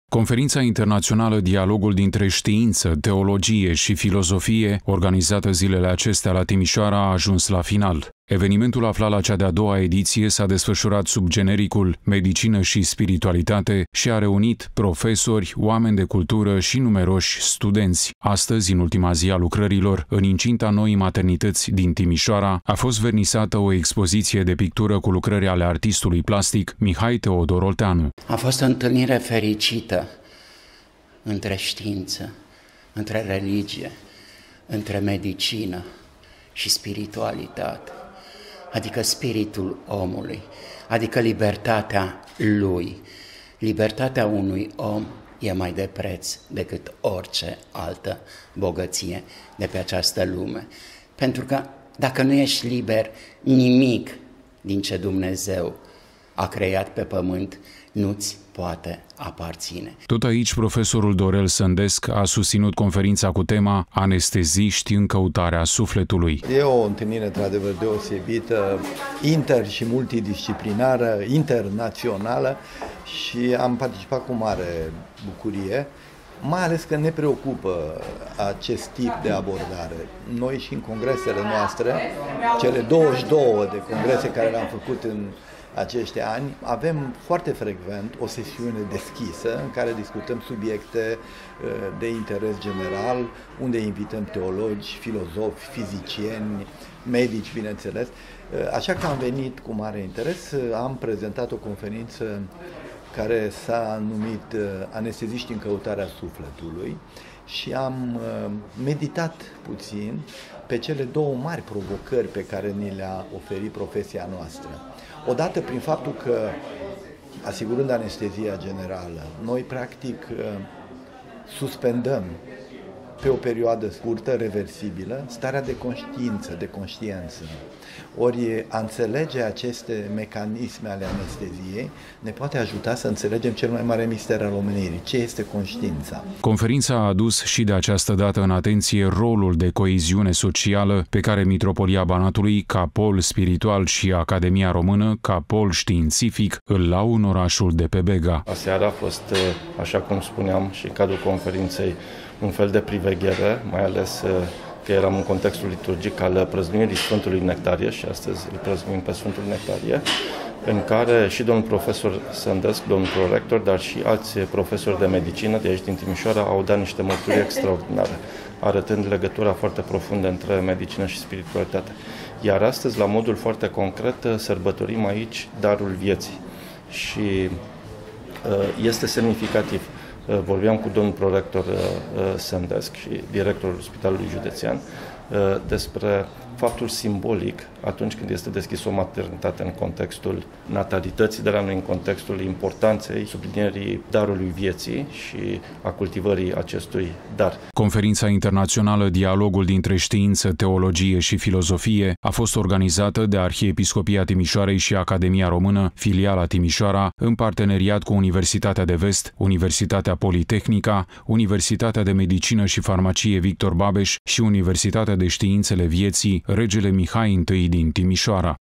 A doua ediție a Conferinței „Dialogul dintre Știință, Teologie și Filozofie”
Conferința Internațională „Dialogul dintre Știință, Teologie și Filozofie”, organizată zilele acestea la Timișoara, a ajuns la final. Evenimentul aflat la cea de-a doua ediție s-a desfășurat sub genericul „Medicină și Spiritualitate” și a reunit profesori, oameni de cultură și numeroși studenți. Astăzi, în ultima zi a lucrărilor, în incinta noii maternități din Timișoara a fost vernisată o expoziție de pictură cu lucrări ale artistului plastic Mihai Teodor Olteanu.